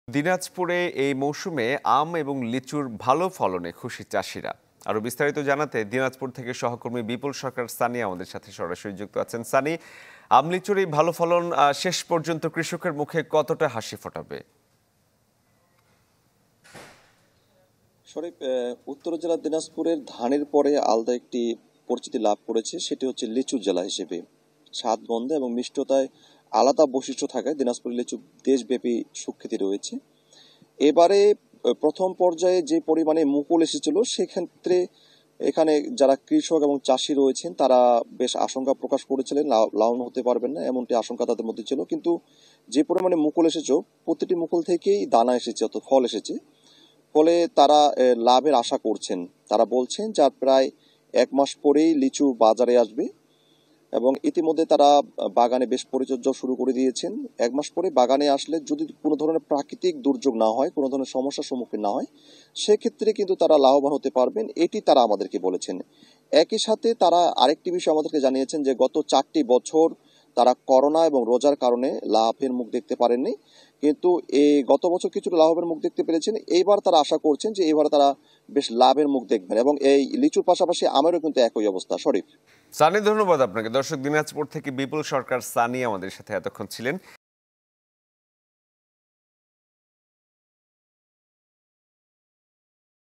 দিনাজপুরে আম-লিচুর ভাল ফলনে খুশি চাষীরা | Dinajpur | Channel 24
»» Office Address:
Bangladesh.